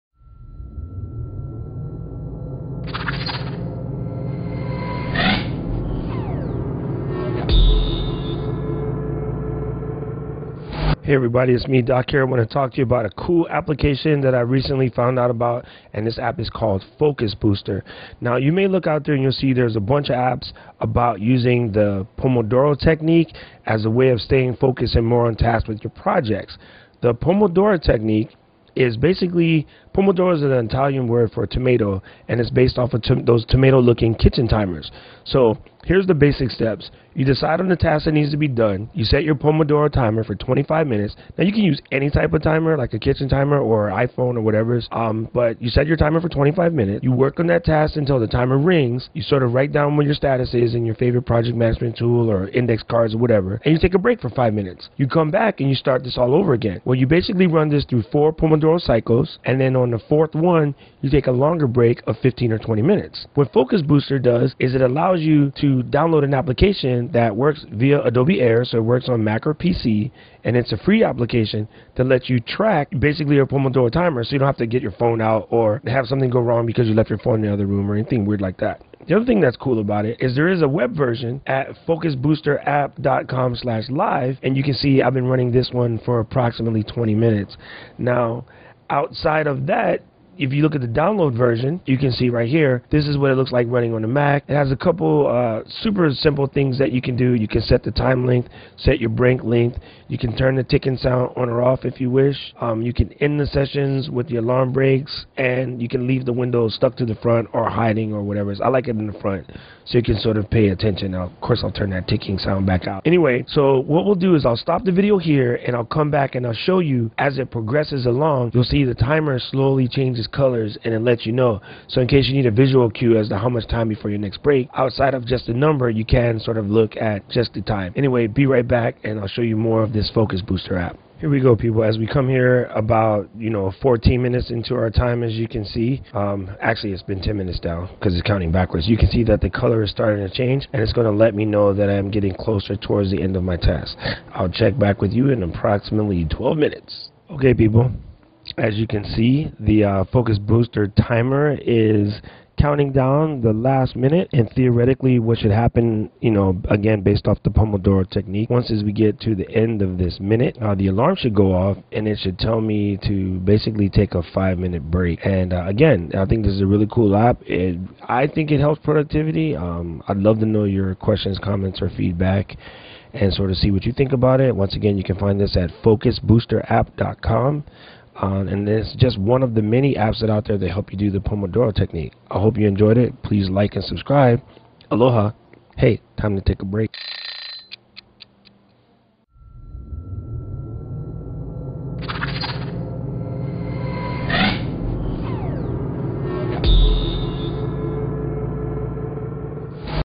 GSD(GTD) with Focus Booster App - Review and Tutorial
Recently I found out about a really cool app designed to help you increase your productivity. Focus Booster like many productivity apps out there are based on the "Pomodoro technique". Pomodoro is a time management technique created in the 1980s by Francesco Cirillo.  The technique is super simple system to take focused work periods and divide them up into 25 minute intervals and then take scheduled five-minute breaks in between set intervals.

The most important parts of this technique are as follows;

decide on the task that is to be done
set the Pomodoro timer for 25 minutes
focus on that task until the timer rings, of update status of that task in your task management program ( I recommend Tracky.com)
take a short break for 5 min. Really get up and stretch this legs and get the circulation flowing
and for every 4 Pomodoro cycles take a longer break (15 to 20 minutes)

Focus Booster unlike most of the other Pomodoro timer apps out there this one is totally free, works on Mac, PC and Linux. Focus Booster also has an online Web app version

In this video I give you a quick review on Focus Booster and I hope you find that it works to help you get more things done.

Connect with Doc